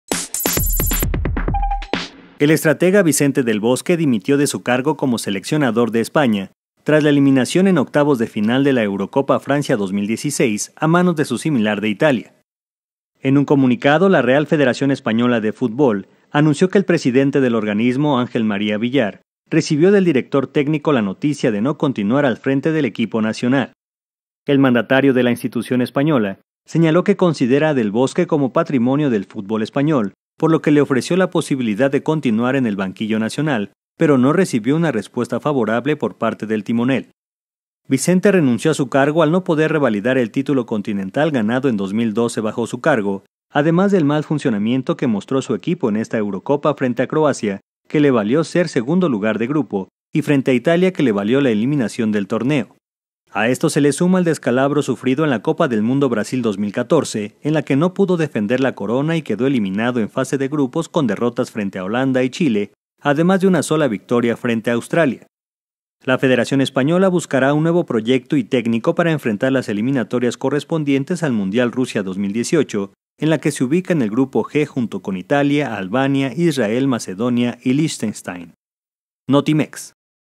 Técnico Vicente del Bosque renuncia como seleccionador de España
El estratega Vicente del Bosque dimitió de su cargo como seleccionador de España, tras la eliminación en octavos de final de la Eurocopa Francia 2016, a manos de su similar de Italia.